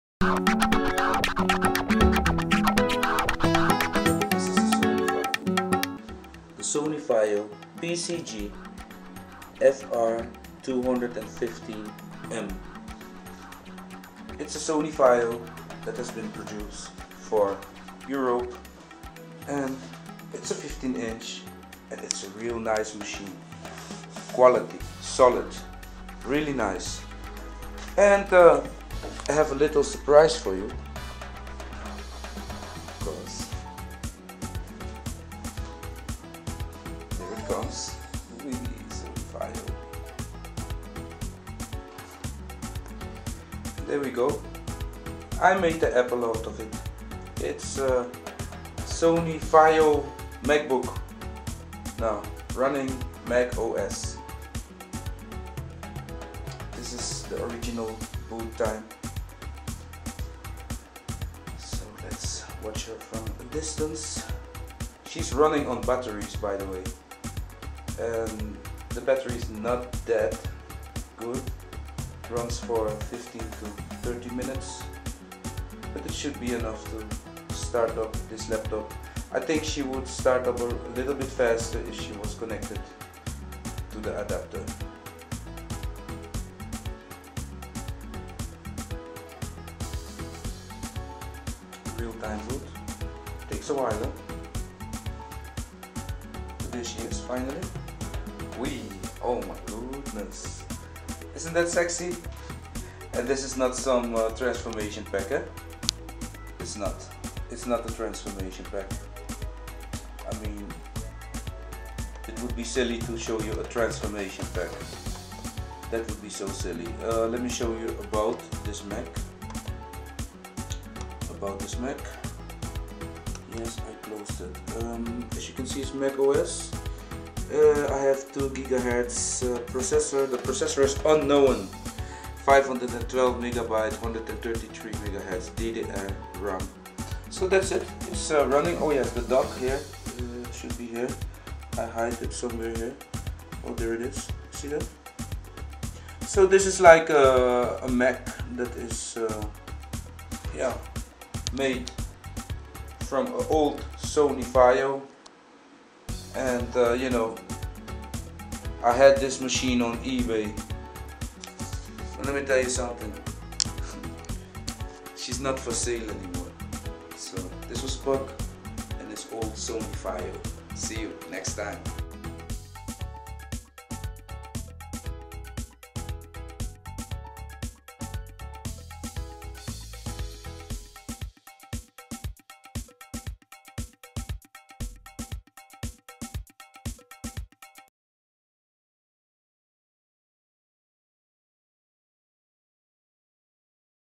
SONY VAIO PCG FR215 - as APPLE MACBOOK - Up and running -
Just a small Update.

Sony Vaio pcgfr215 can be used to test as a macbook
with the right software ...

Amazing project .

Video Channel48
by Puck Darlington

(c)2014